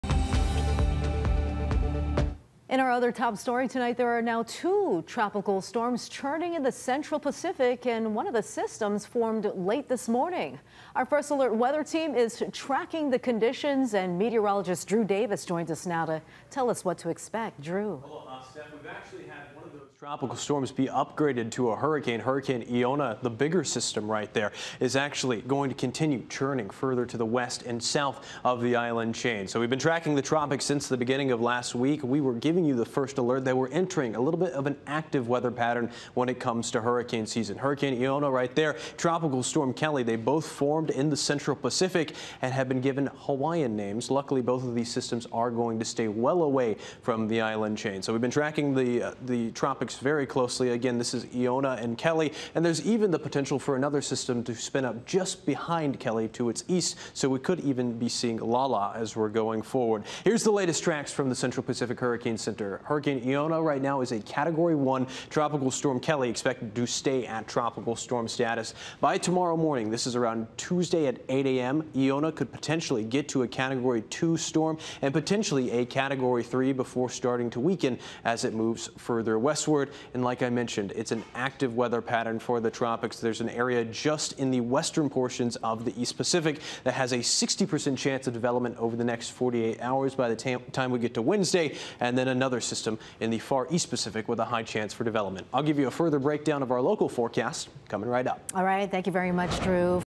Hurricane Iona and Tropical Storm Keli form southeast of Hawaii
Forecasters are now tracking two named tropical cyclones in the Central Pacific, as a newly-formed tropical depression has already strengthened.

Meteorologist Drew Davis breaks down the storm forecasts. Neither storm is expected to impact the island chain.